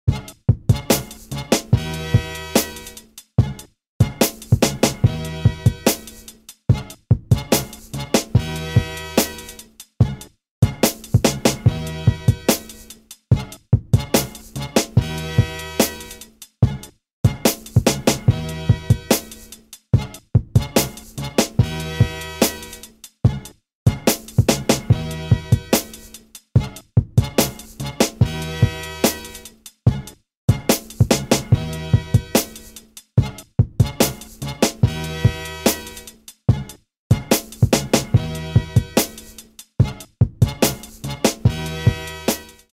Onelinefabet - Graffiti Throwup style - One line alfabet - by Maedist
I've created this font that has only a curve to the right and might be the quickest way to do a graffiti throwup.
Can you do it? It took me some attempts to get it in one time.